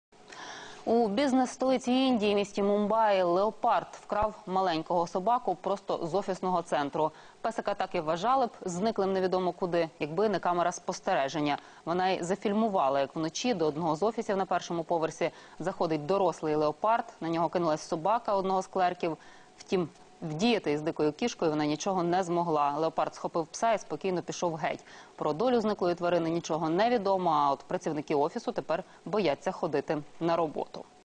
В Мумбаи леопард утащил собаку прямо из офисного цен...
В Мумбаи леопард утащил собаку прямо из офисного центра - Новости. День - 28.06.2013

In Mumbai leopard dragged the dog right out of the office center - news. Day - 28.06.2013